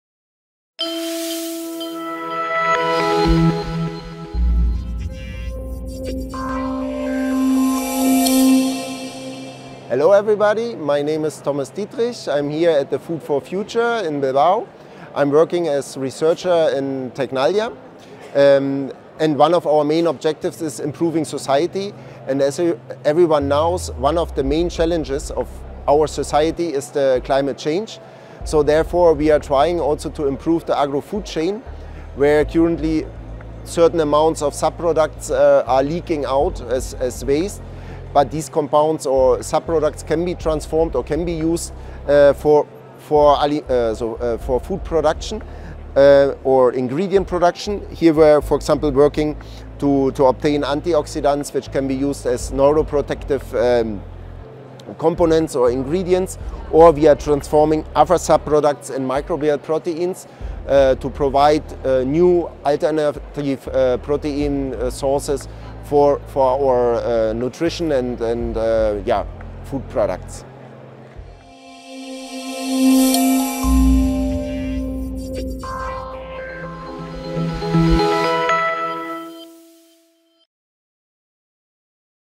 We talk with Thomas Dietrich researcher in TECNALIA
The 2nd edition of FOOD4FUTURE is back as the most revolutionary edition in Technology, innovative solutions and an extensive expo for the FoodIndustry

🔸TECNALIA aims to provide smart solutions for the food industry with innovative technology, in line with market needs.
🔸FOOD4FUTURE, a space for sharing innovation that will transform the food industry

📍 BILBAO EXHIBITION CENTRE (BEC) FOOD 4 FUTURE - EXPO FOODTECH
⏳ 17 - 19